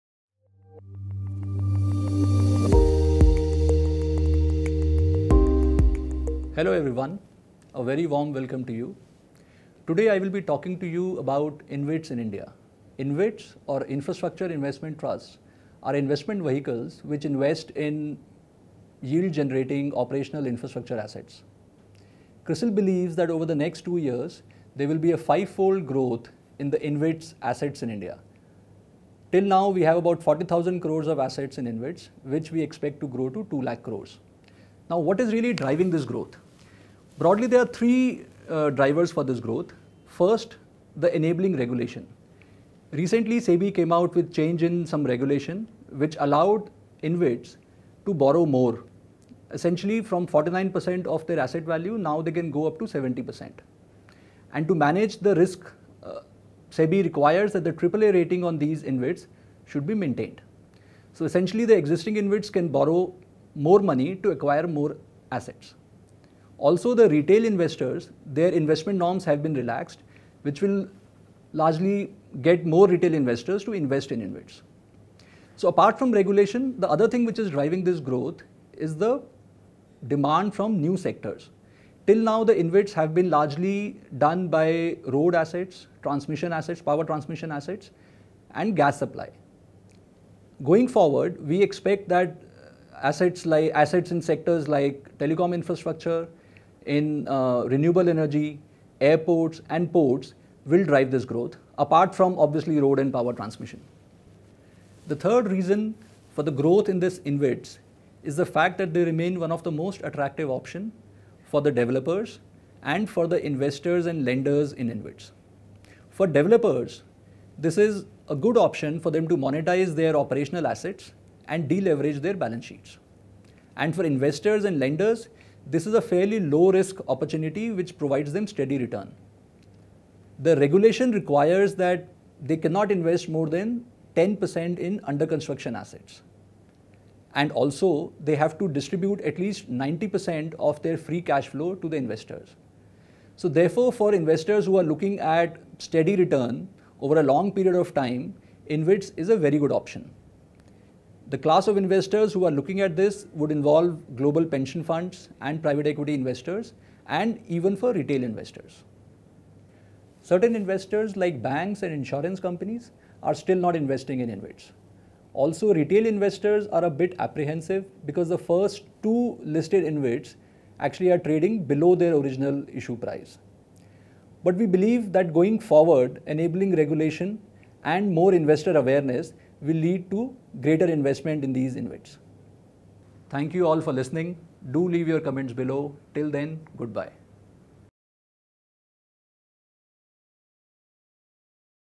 CRISIL TV - InvIT assets to grow five-fold to ₹2 lakh crore
Sachin Gupta, Senior Director, CRISILRatings, says Infrastructure investment trust (InvIT) assets to grow five-fold to ₹2 lakh crore. Regulatory changes make InvITs attractive to investors with low-risk appetite.